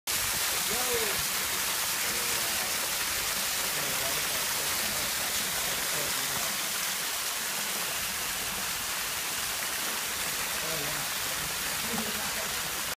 Main falls of Upper Big Tujunga
Footage from our adventure to Upper Big Tujunga.